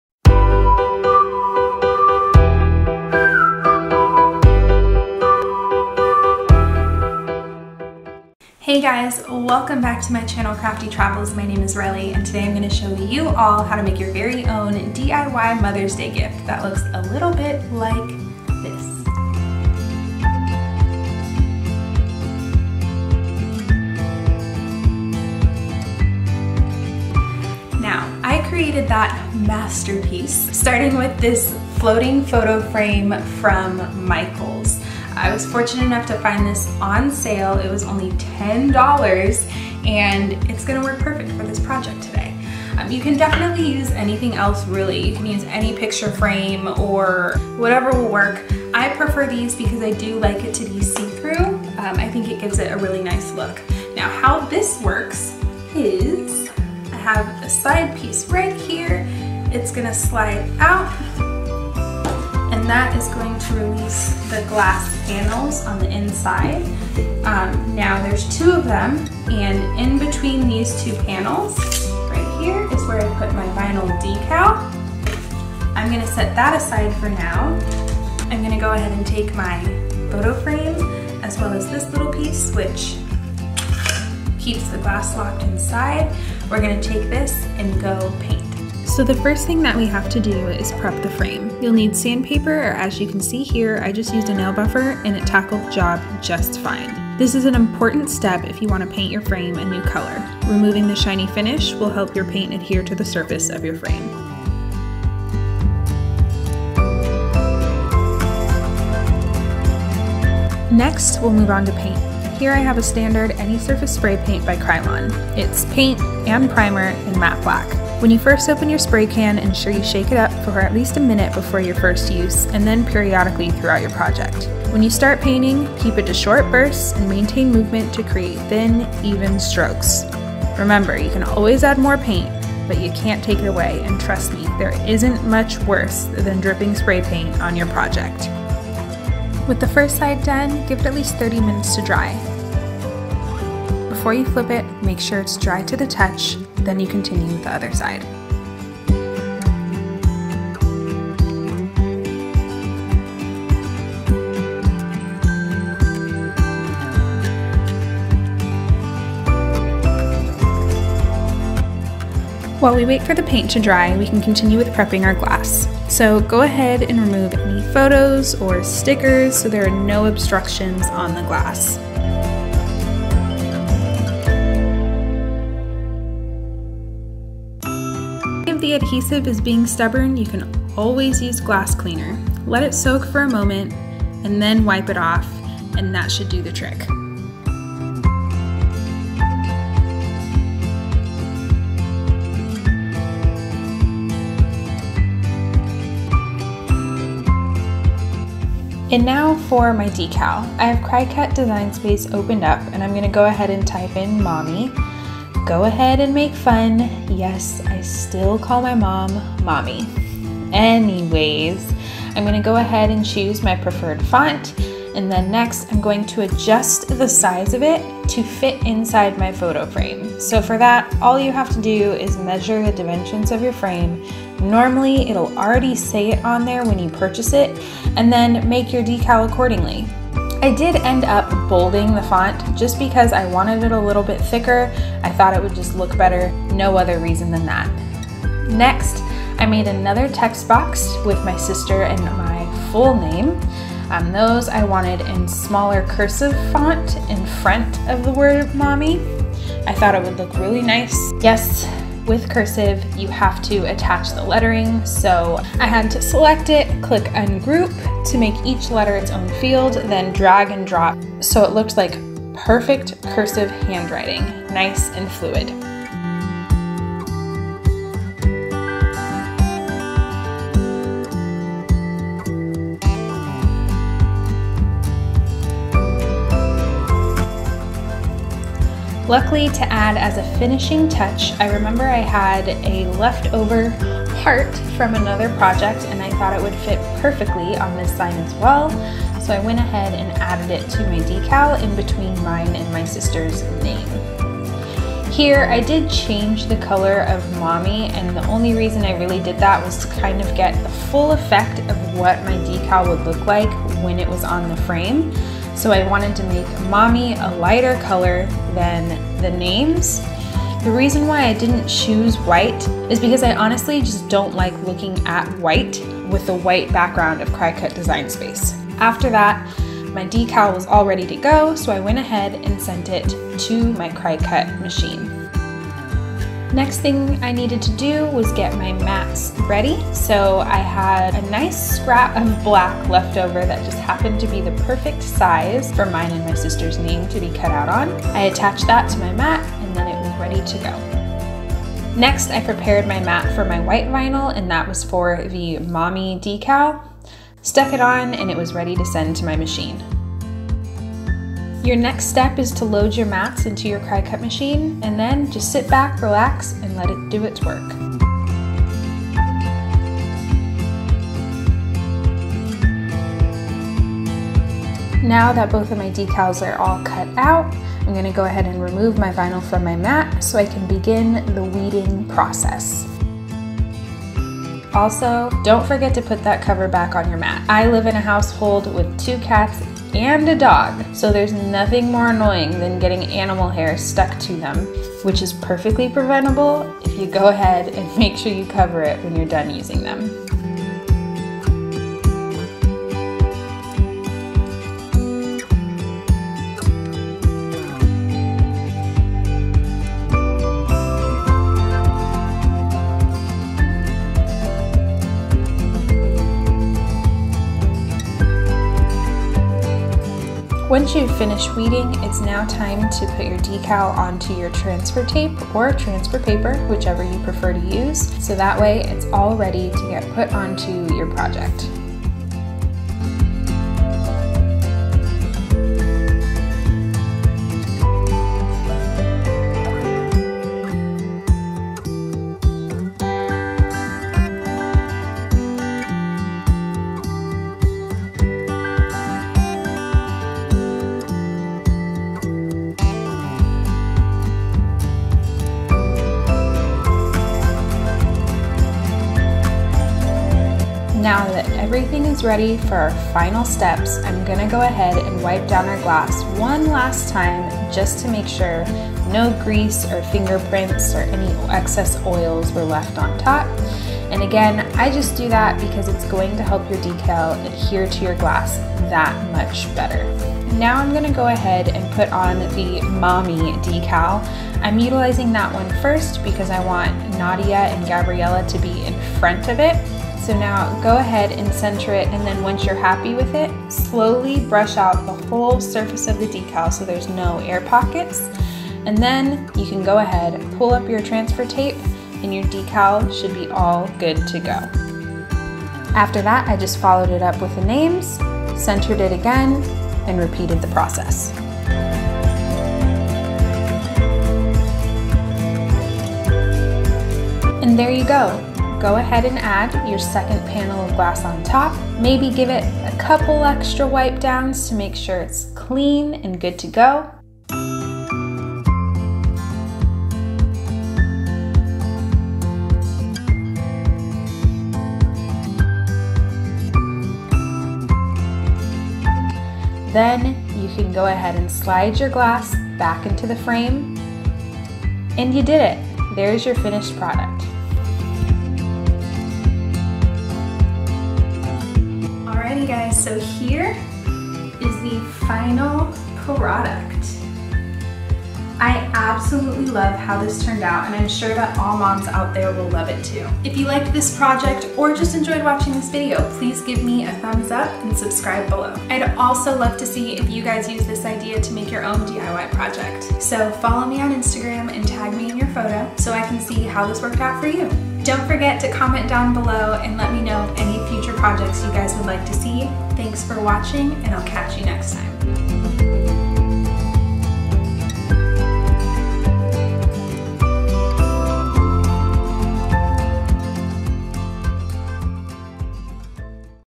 DIY Mother's Day Gift
Hey Everyone! This is the gift I made for my mom this Mother's Day, and I hope it can inspire you all on your own projects. Its a floating photo frame which I turned into a sign with mine and my sister's names inside.

To recreate this project you'll need:
-photo frame
-sand paper (or a nail buffer worked fine for me)
-spray paint (optional. although any paint for the frame will work)
-decal (a cricut machine does make the process easier, however you can always hand cut a design as well)
-transfer tape for vinyl
Time Stamps
Prepping frame - 1:30
Painting frame - 1:50
Prepping Glass - 2:45
Creating Decal - 3:20 (Desing Space, Cricut, & Weeding)
Adding Decal to Frame - 7:30
Finishing Touches - 8:50
Finished Product - 9:00

Spray Paint:
iVyne Weeding Kit:
Transfer Paper:
Cricut Machine:
Vinyl Starter Pack: